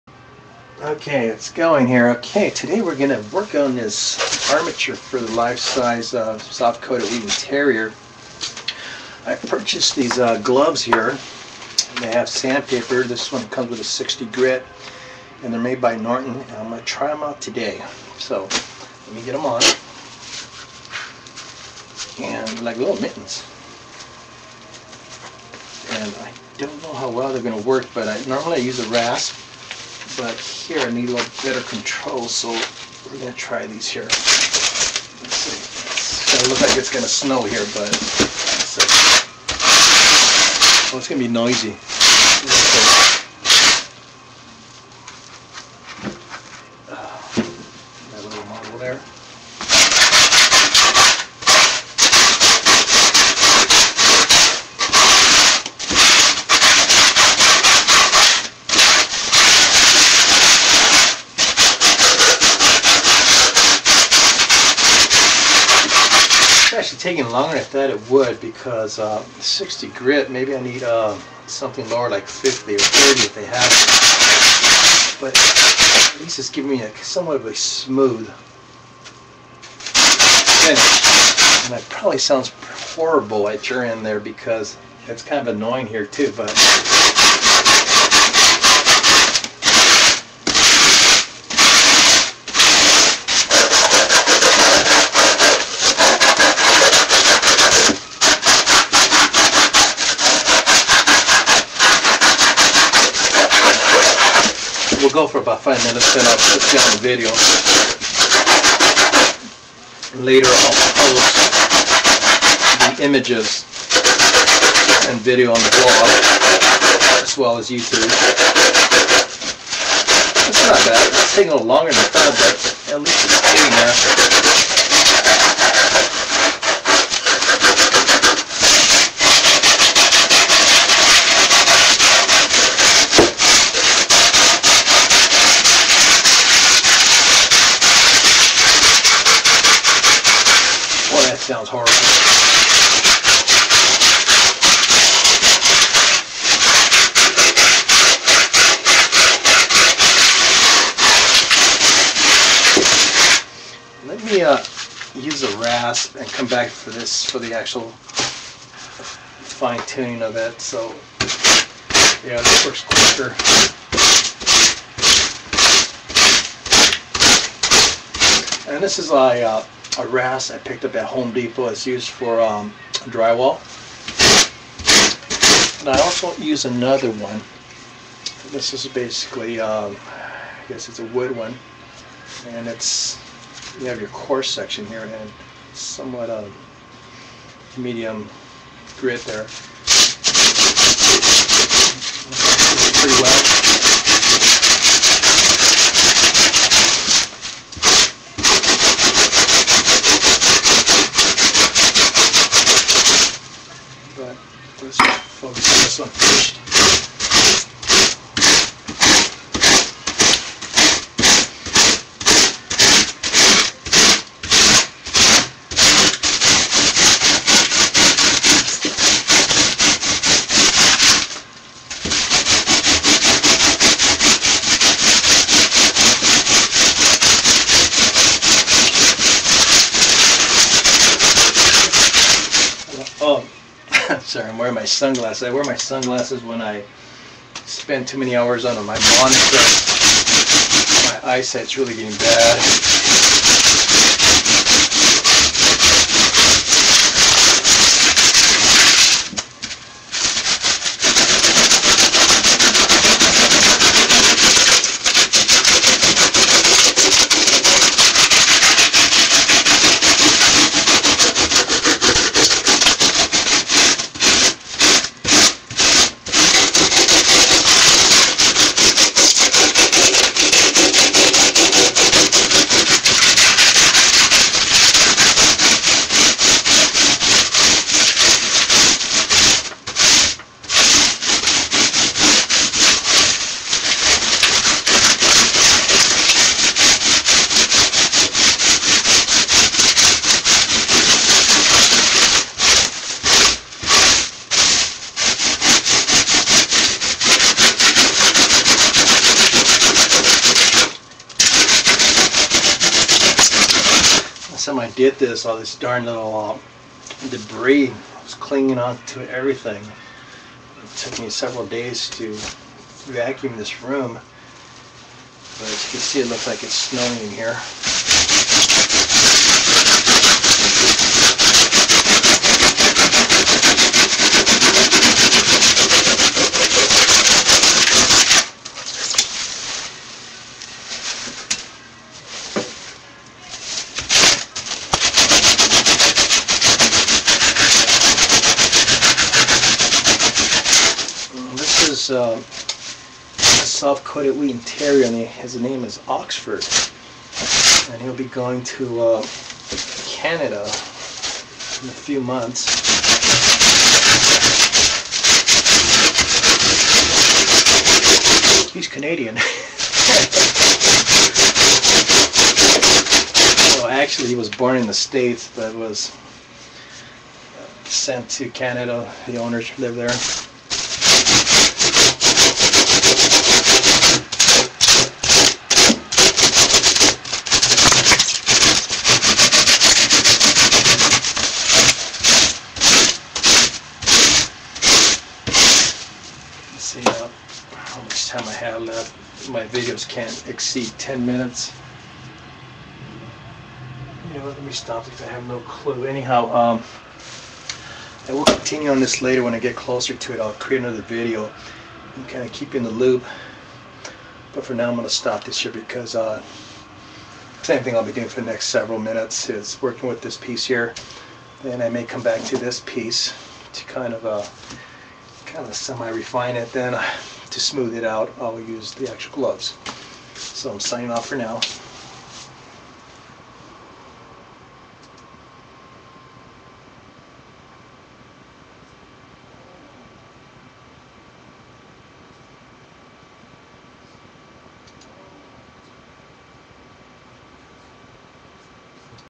sculpting the foam armature for the life-size wheaten terrer
to view more of my art
using a rasp and sanding gloves to shape the Styrofoam armature. The foam is the armature for the clay in the next phase of the dog sculpture.